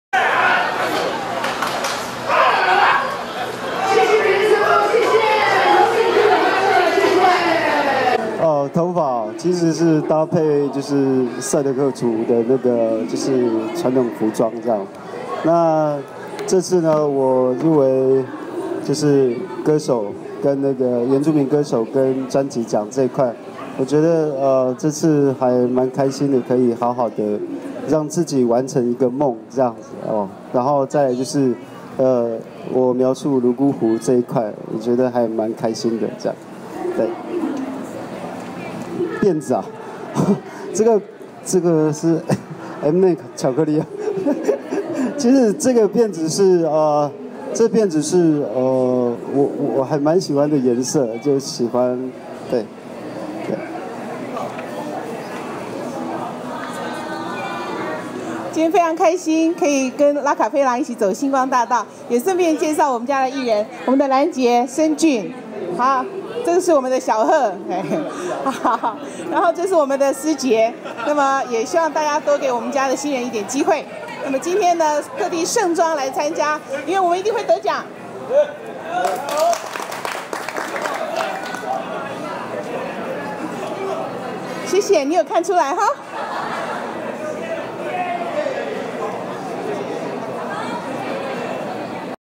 【第25屆流行音樂金曲獎星光大道】拉卡飛琅、于美人、林思杰、曾治豪、森竣、陳藍杰
06/28  台北小巨蛋